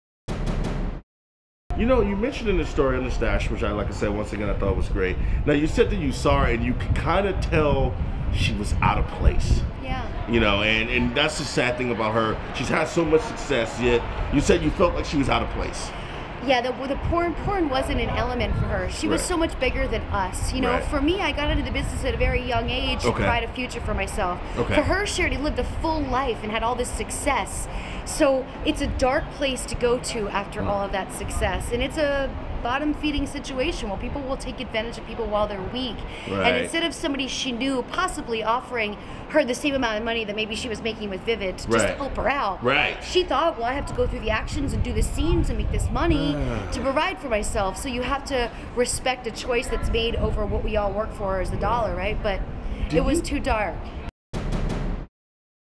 Lisa Ann- Chyna Didn't Belong In Porn...'She Was Bigger Than Us' | TMZ Sports
Chyna was a larger than life icon who should've never done porn ... that's according to one of her co-stars Lisa Ann ... who says the bang biz was too dark a place for the late WWE star.